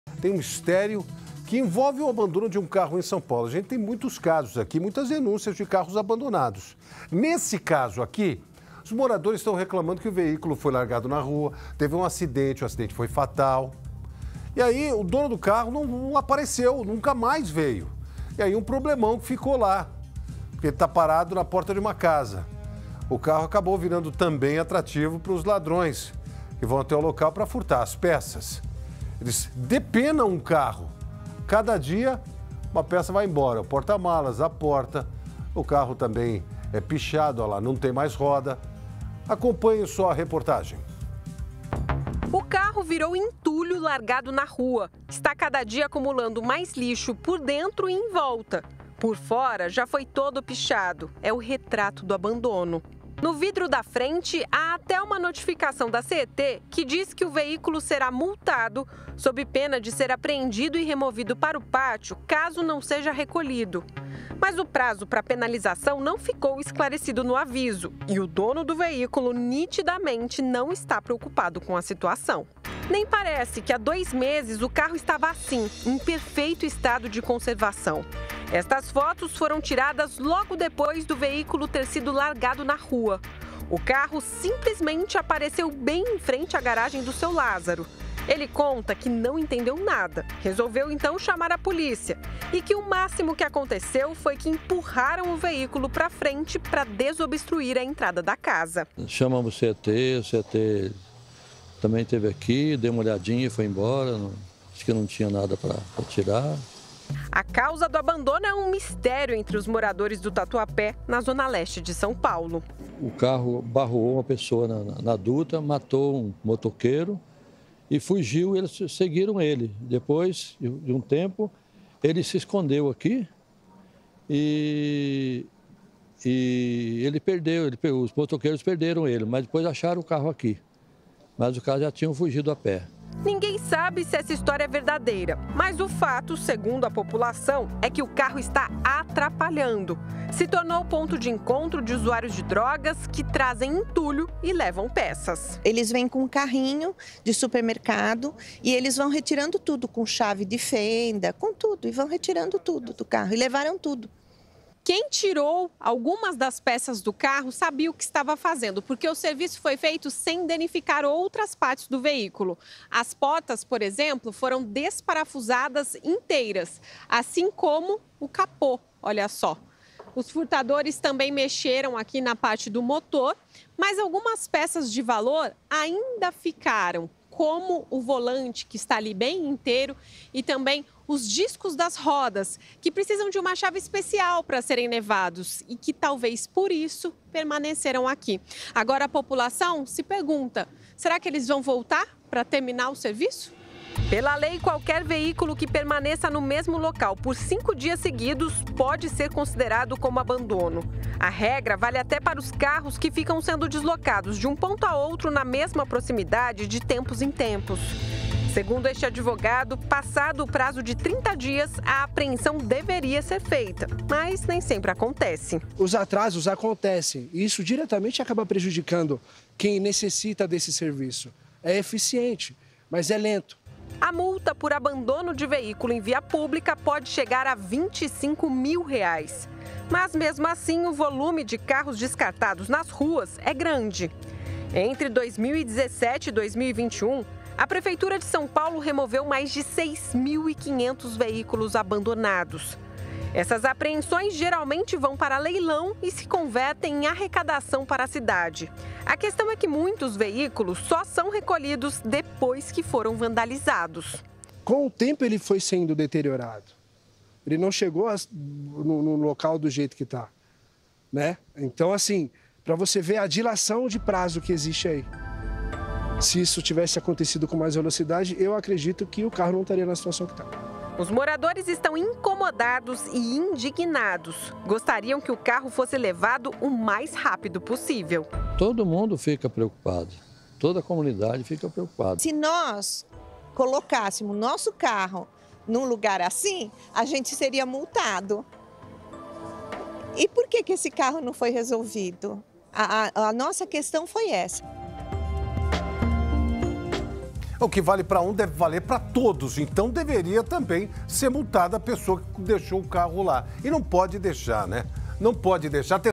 Carro envolvido em acidente com morte é abandonado em rua de SP
Moradores reclamam de um carro abandonado em uma rua de São Paulo após um acidente com morte. Veículo virou chamariz para ladrões, que vão até o local para roubar peças.